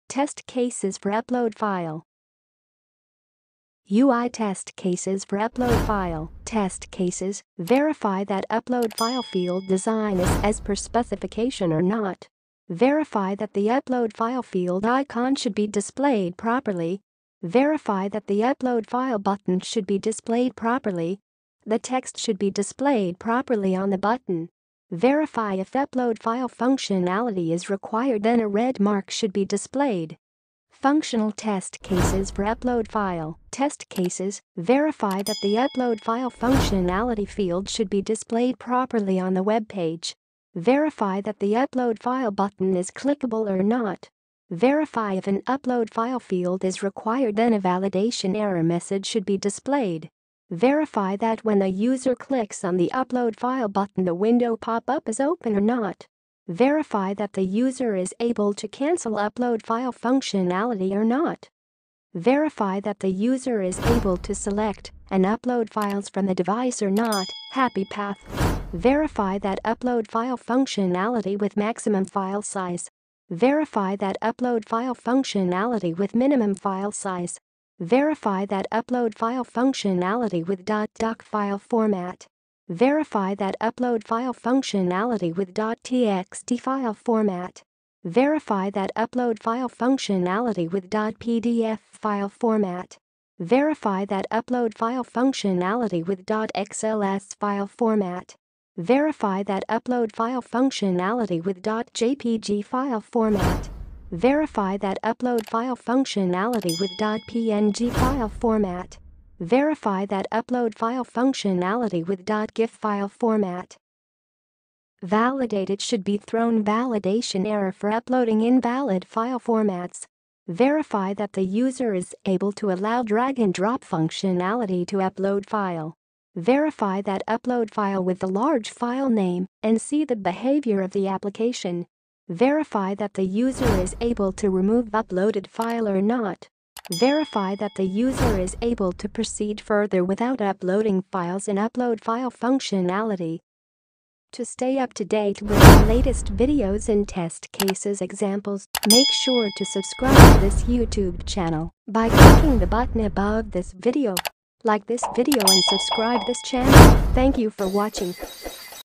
Test Cases For Upload File | UI and Functional Test Cases For Upload File
Test cases for upload file | Test scenarios for upload file :-

How to write test cases for upload file:-

- UI test cases for upload file
- Functional test cases for upload file

Test cases for upload file are one of the most commonly asked interview questions in software testing.

Chapters:

0:00 start
0:03 UI test cases for upload file
0:30 Functional test cases for upload file